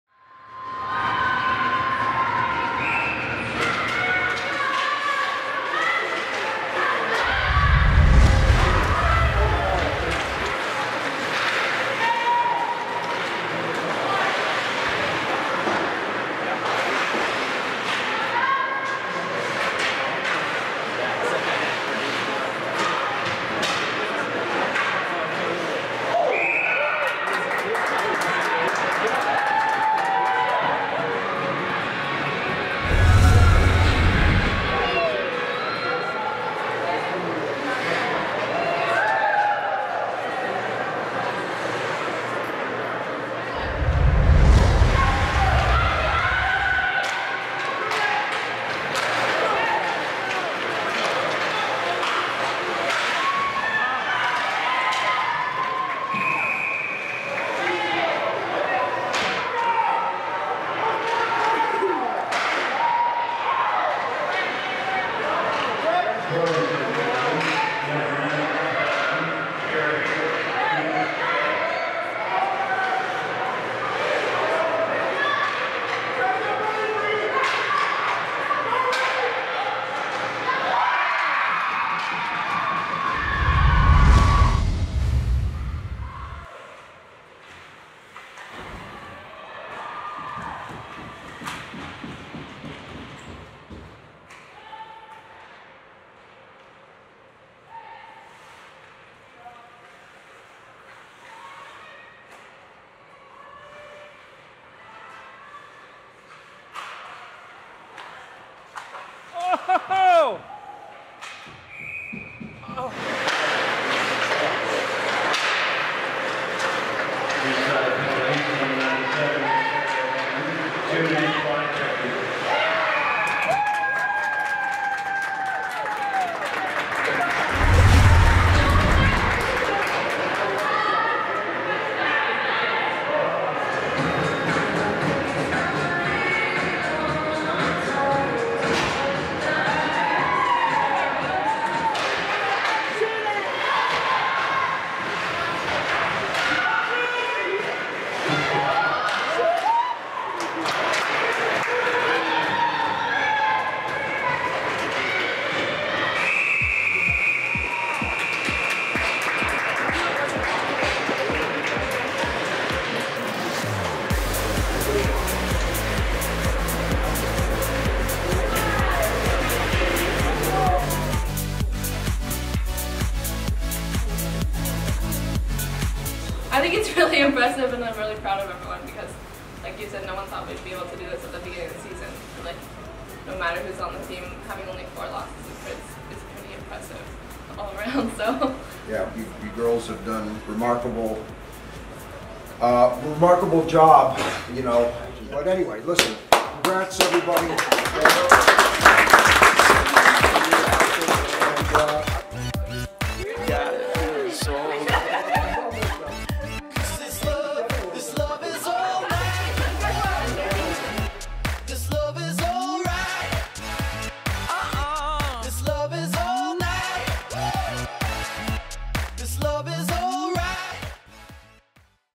Game Highlights vs Toronto Leaside Wildcats March 18th, 2023
2022/2023 Regular Season

Goals scored by:

#97 Keira Healey, #44 Bryn Prier, #21 TJ Flores

#14 Teagan Gray, #93 Avery Muhn, #97 Keira Healey